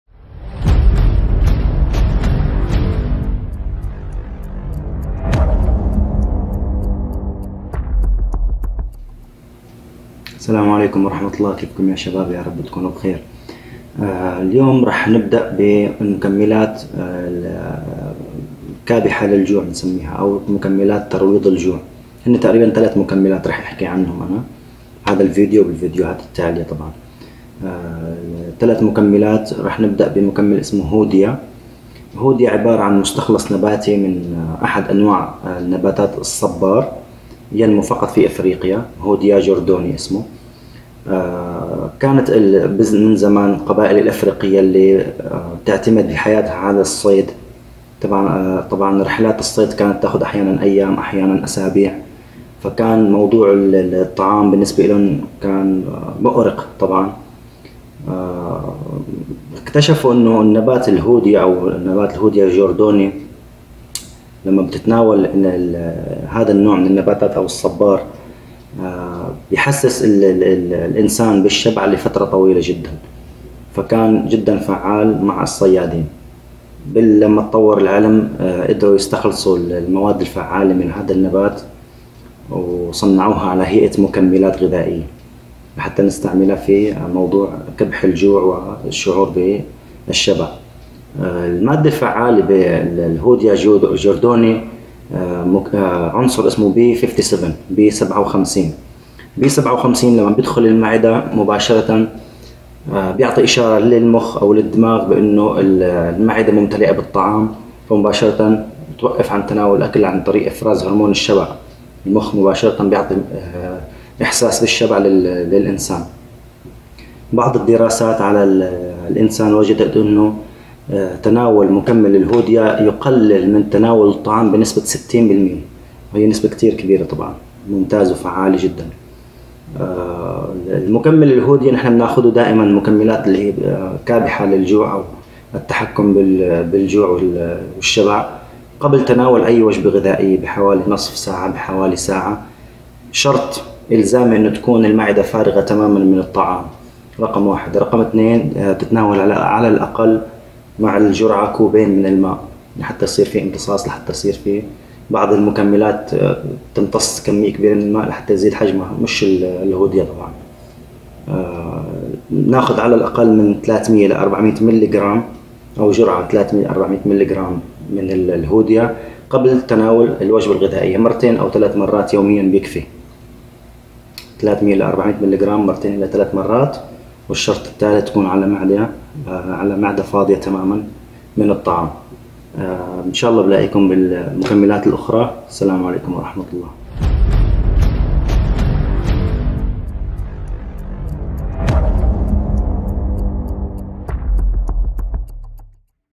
هل تجد صعوبة بالتحكم بالجوع والشهية لديك؟ لدينا الحل : هوديا جوردوني Control your appetite : Hoodia
مكملات هوديا نباتية المنشأ وهي فعالة و آمنة تماما للذين لا يستطيعون التحكم بالشهية للطعام